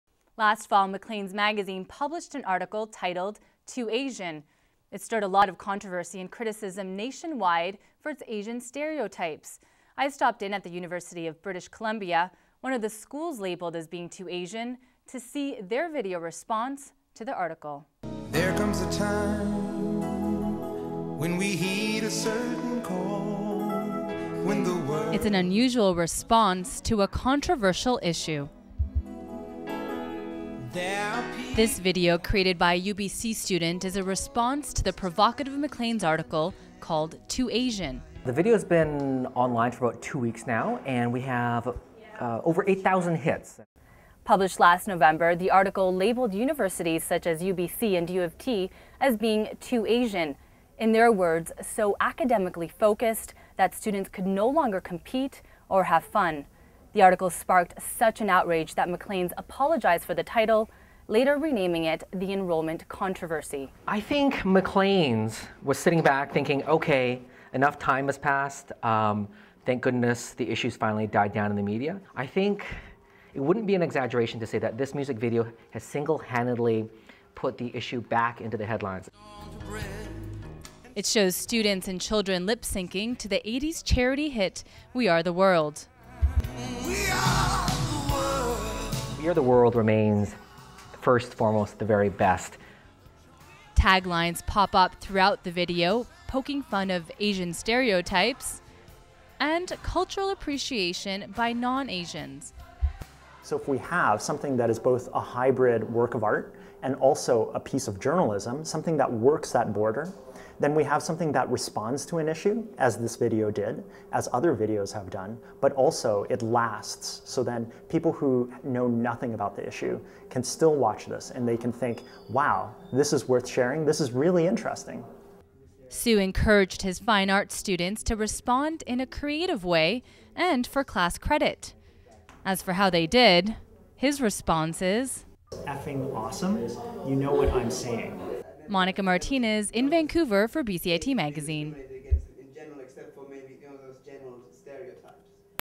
UBC Too Asian response - BCIT Magazine, Feb. 10, 2011
Last November, Maclean's magazine published a provocative article titled :Too Asian?". It stirred a lot of controversy and criticism nationwide for its Asian stereotypes. UBC was one of the schools labelled as being "too asian." Reporter Monica Martinez talks to a UBC student who created a video response to the article.

shooter/editor/reporter - Monica Martinez
BCIT Magazine, Feb, 10, 2011